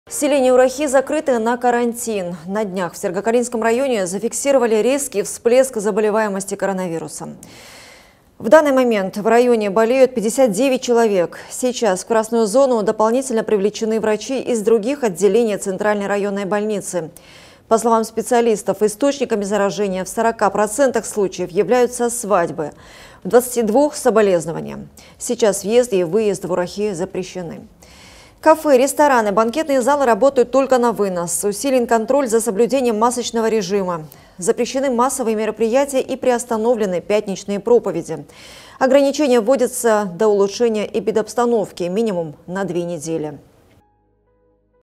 Село в Сергокалинском районе закрыли на карантин из-за вспышки COVID-19
Селение Урахи закрыто на карантин. На днях в Сергокалинском районе зафиксировали резкий всплеск заболеваемости коронавирусом.

В данный момент в районе болеют 59 человек. Сейчас в «красную зону» дополнительно привлечены врачи из других отделений центральной районной больницы. По словам специалистов, источниками заражения в 40% случаев являются свадьбы, в 22% - соболезнования.

Сейчас въезд и выезд в Урахи запрещены. Кафе, рестораны, банкетные залы работают только на вынос. Усилен контроль за соблюдением масочного режима. Запрещены массовые мероприятия и приостановлены пятничные проповеди.

Ограничения вводятся до улучшения эпидобстановки, минимум на две недели.